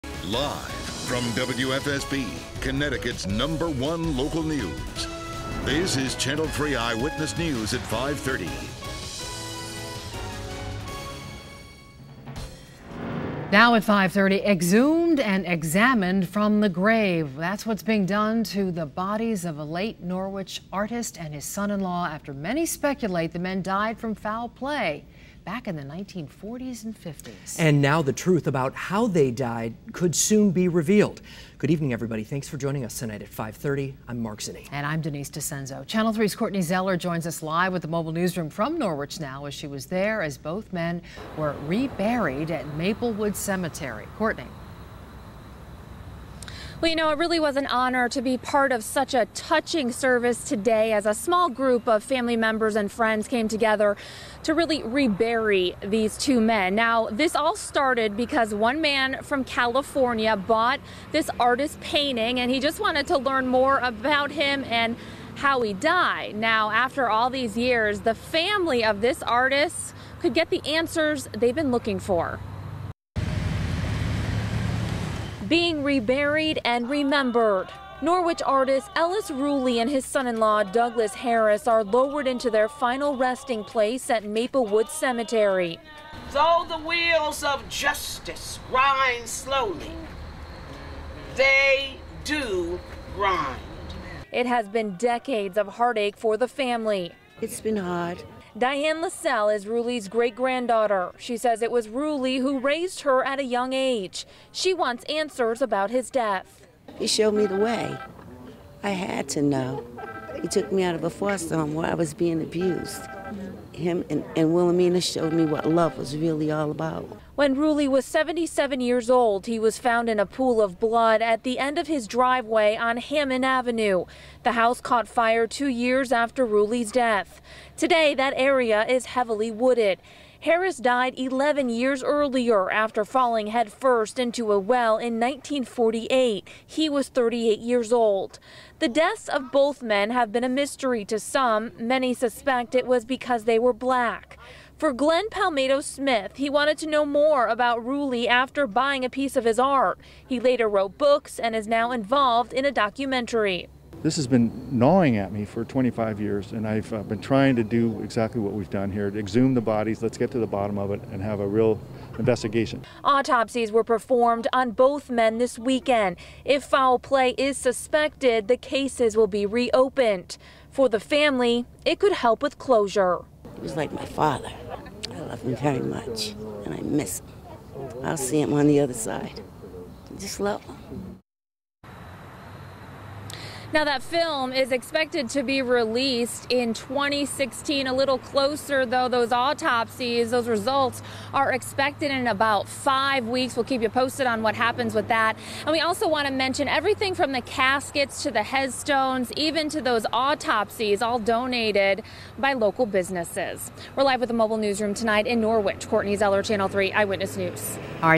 Courtney Live Shot
The reinterment of the bodies of Ellis Ruley & Douglas Harris. Famed forensic pathologist, Dr. Michael Baden, performed the examinations on both victims. Labeled at the time by authorities as death by alcohol and falling, (Ruley) and accidental drowning, (Harry), the final medical examiner's findings will hopefully dispel these "rushes to judgement".